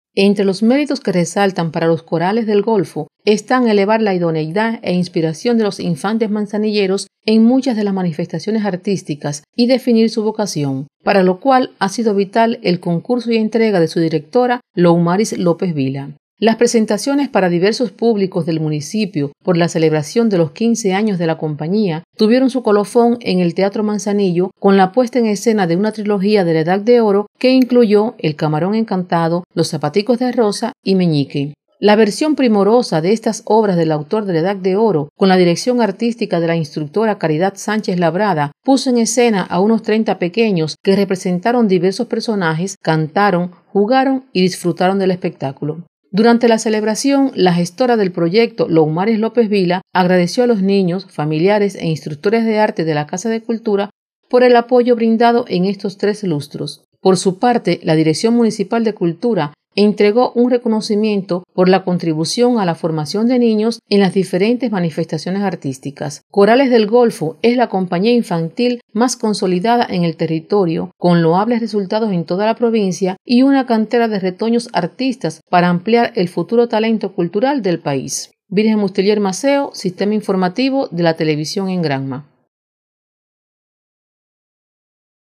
Compañía infantil Corales del Golfo muestra solidez de su quehacer
La Compañía manzanillera Corales del Golfo cumple 15 años de fundada, para celebrarlo desarrollaron una jornada de presentaciones. La agrupación infantil muestra solidez creativa gracias al empeño de instructores de arte, especialistas y técnicos pertenecientes al movimiento de artistas aficionados.